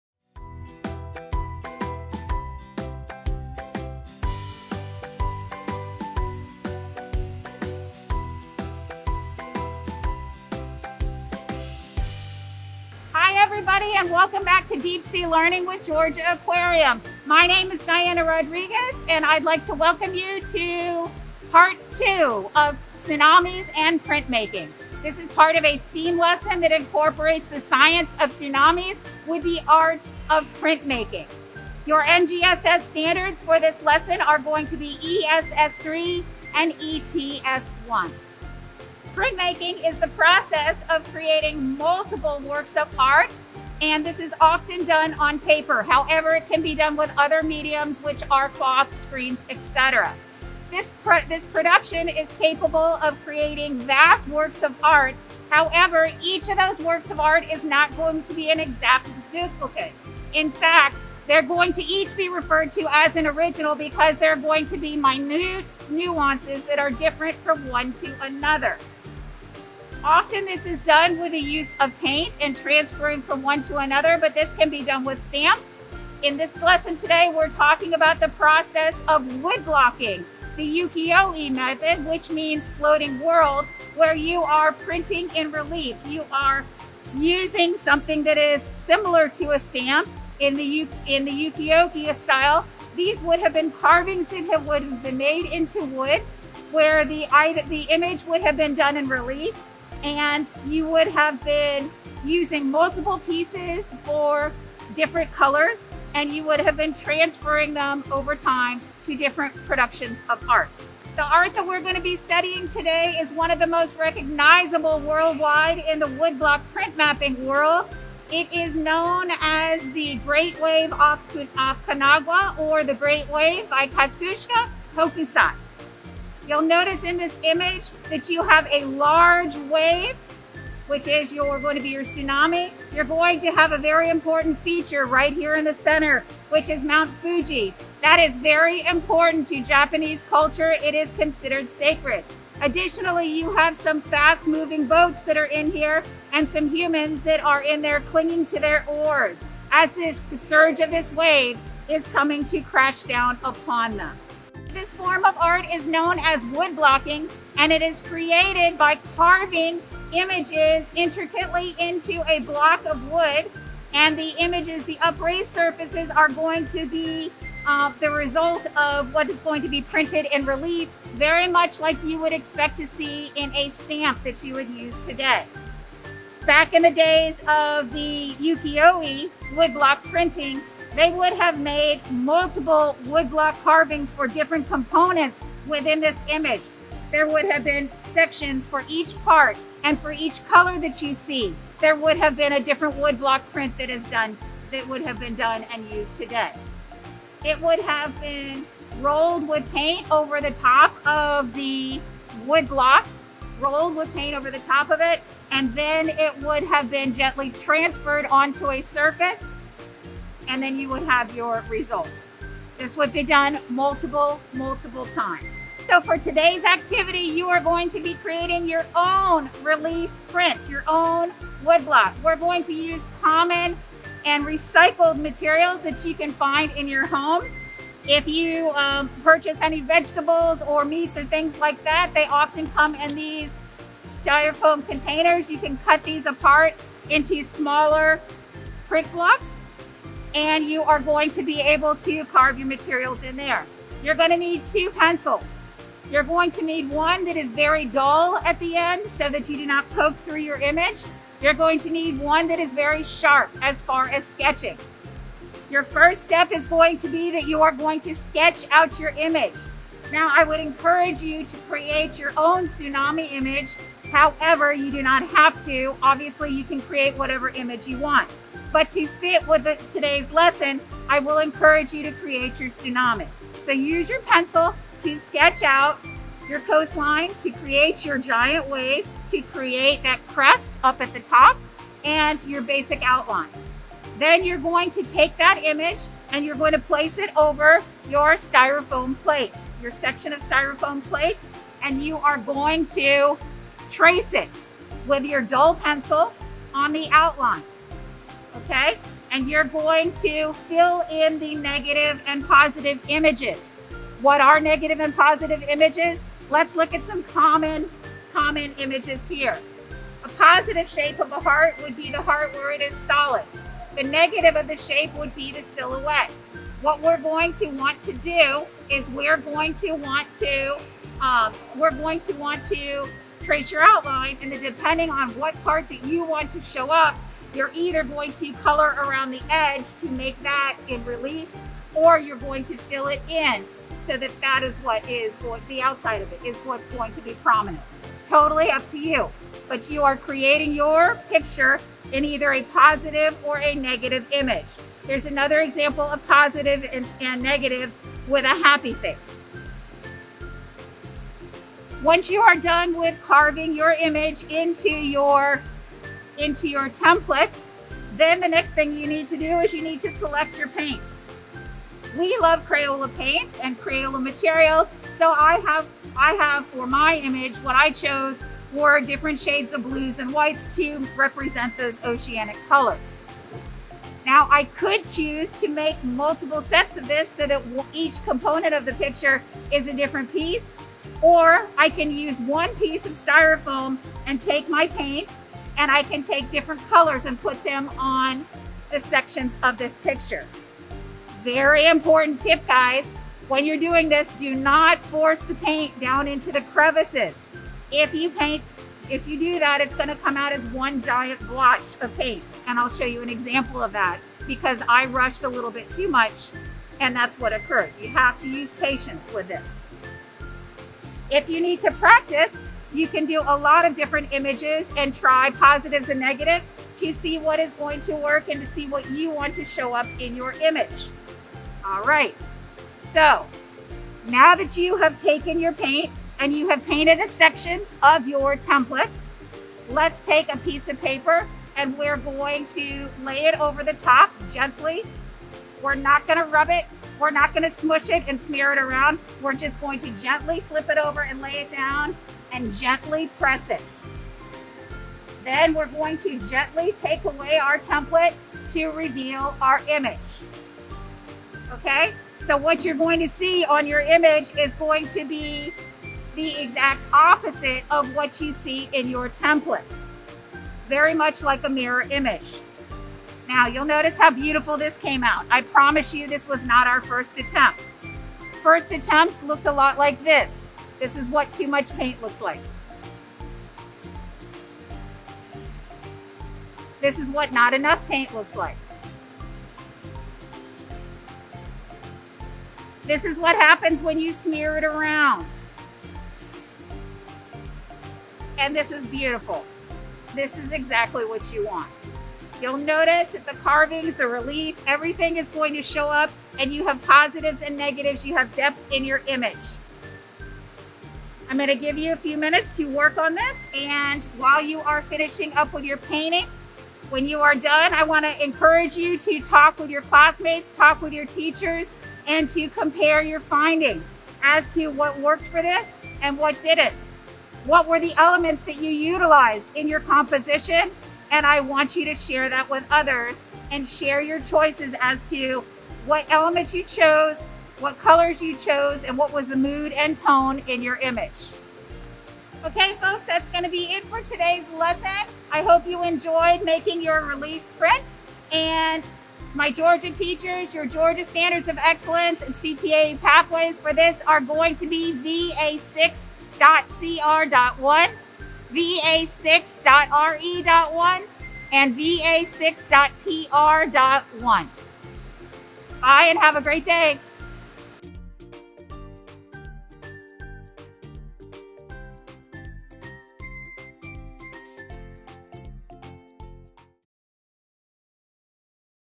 Deep Sea Learning: Tsunamis Part 2
Deep Sea Learning with Georgia Aquarium's Education Department
Episode 23: Tsunamis Part 2